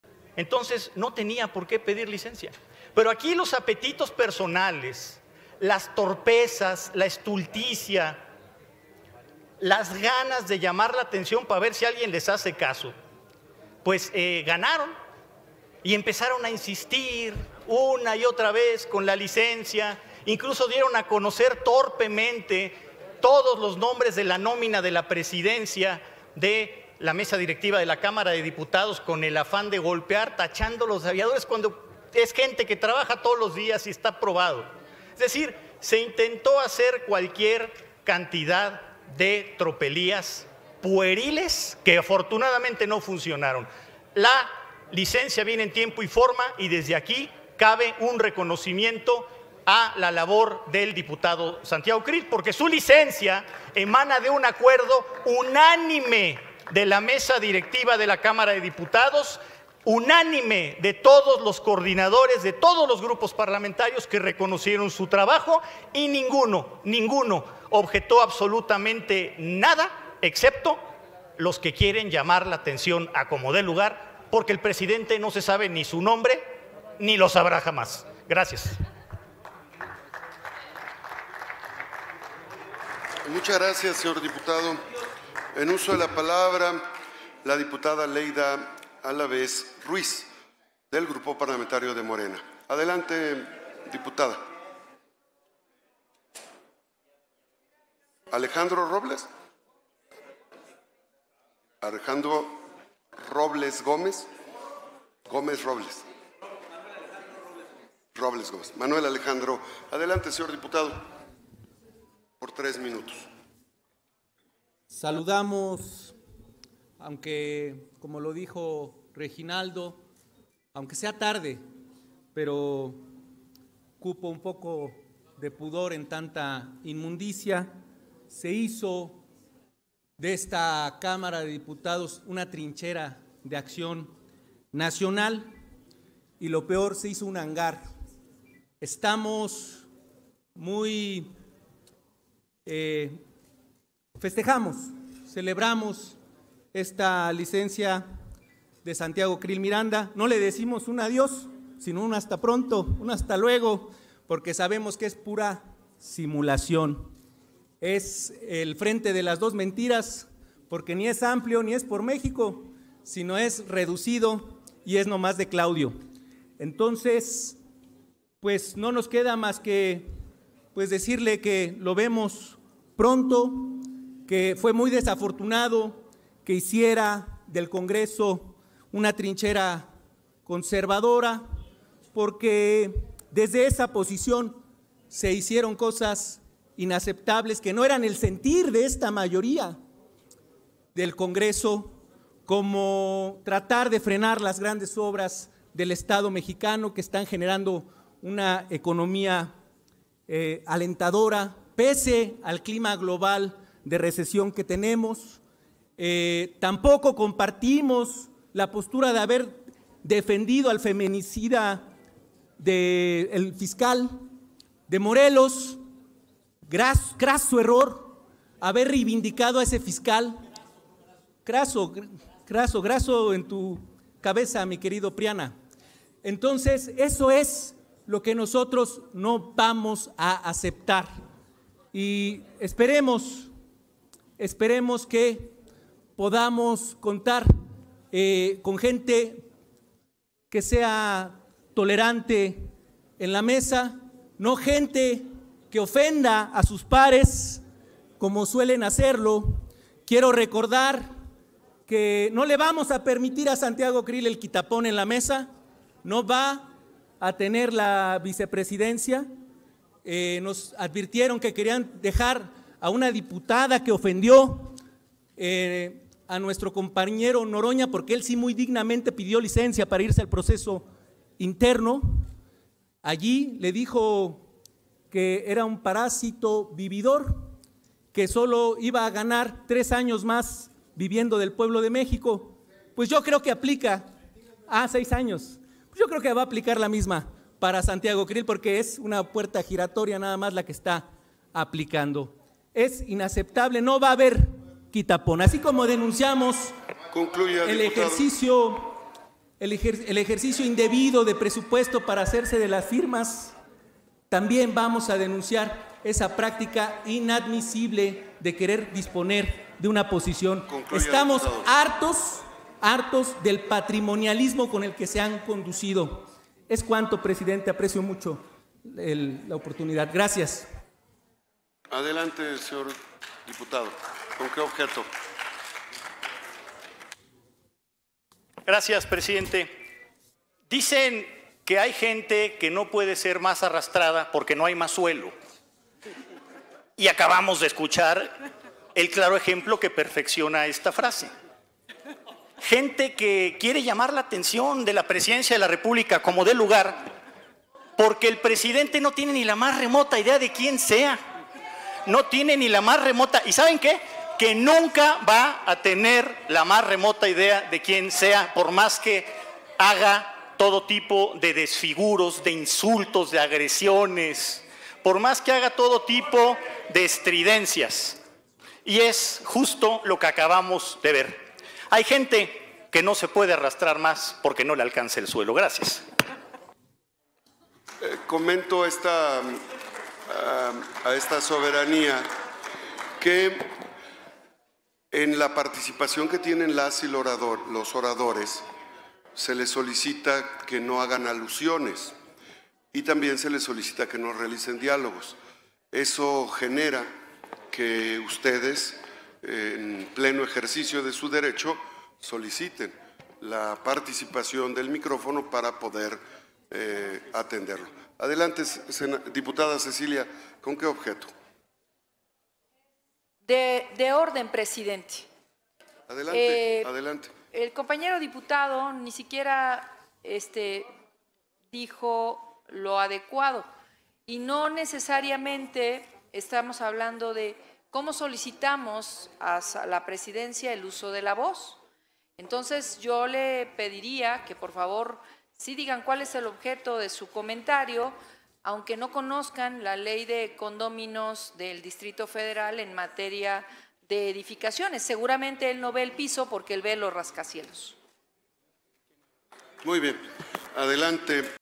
ENCONTRONAZO entre PANISTAS y MORENISTAS por RENUNCIA de CREEL
Diputados y senadores del PAN y Morena enfrentaron posiciones luego de la solicitud de licencia de Santiago Creel, aspirante presidencial del PRIAN.